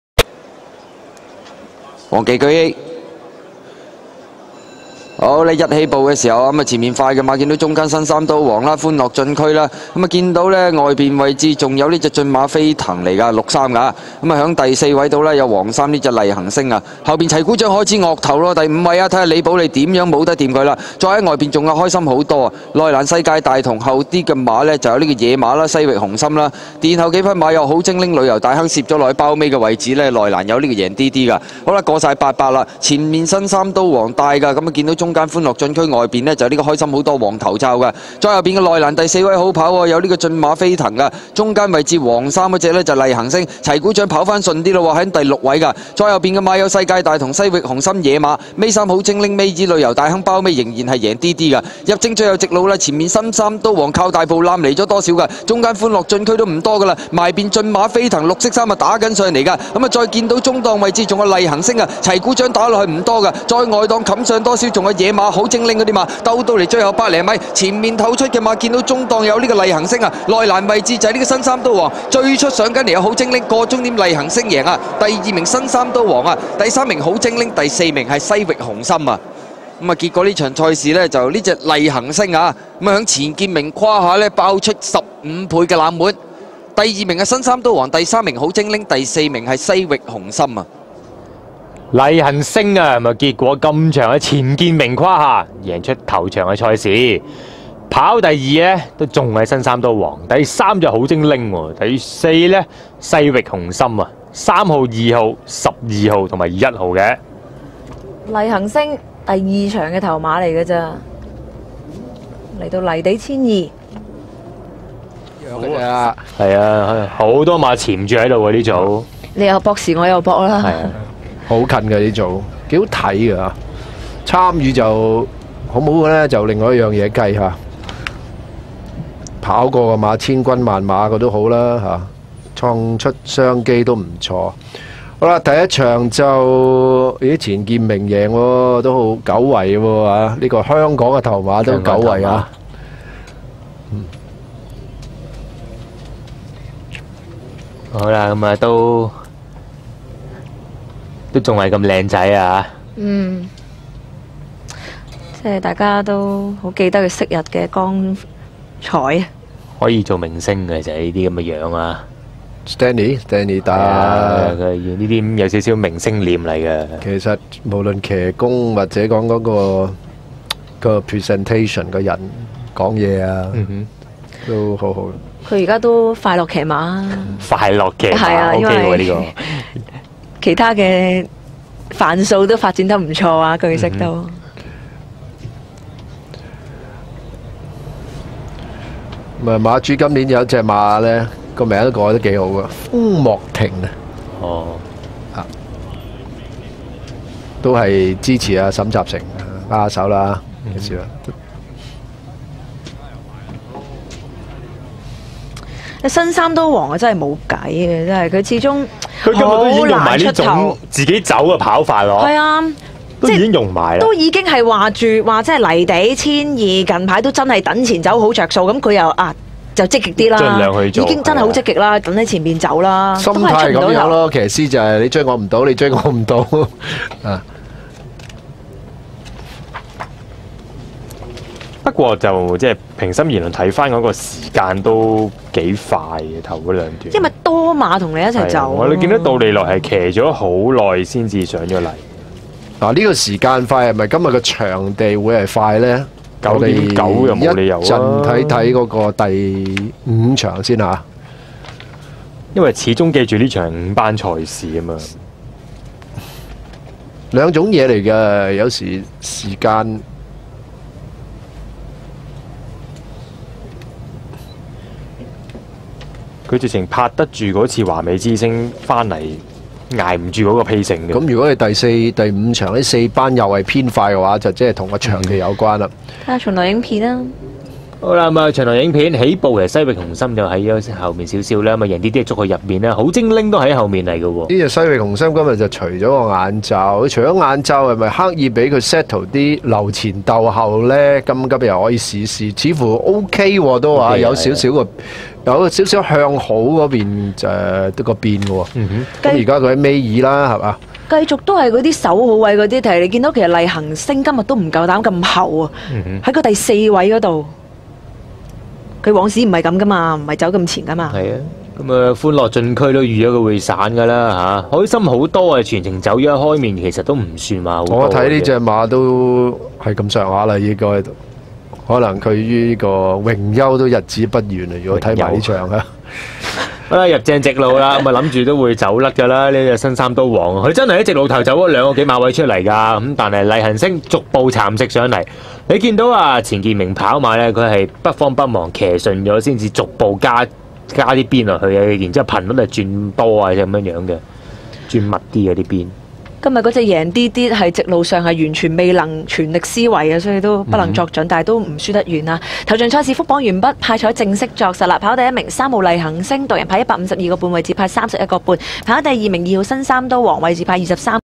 2011/12 (461) - Planet (厲行星) - 錢健明 (久遺了在香港贏的頭馬)
Class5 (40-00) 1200M Dirt - Time: 1.10.70

Jockey : KM Chin (錢健明)
Trainer : CS Shum (沈集成)
Owner : Mr & Mrs Yiu King Chuen and Chris Yiu Chun Hang (姚景存先生及夫人與姚振恒)

2012/03/10 - Hong Kong - Sha Tin (461)